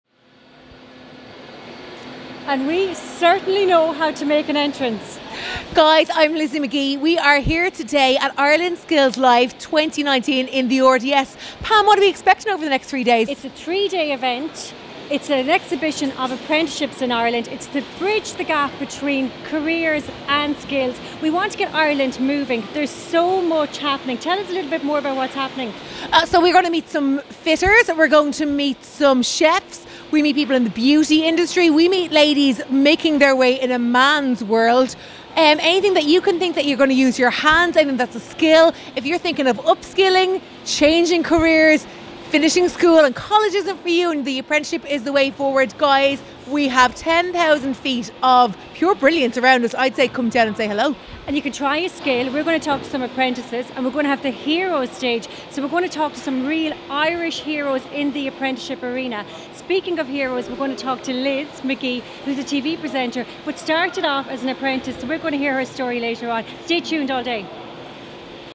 Ireland Has The Skills - Social TV
Social TV is live with Lizzie McGee & Pam Finn for Hashtag Media. Broadcasting live at Ireland Skills Live over three days where we will see Ireland's Top Apprentices & Skill People compete to become Ireland's best. An expo of education for careers and most importantly the future of Irish Industry.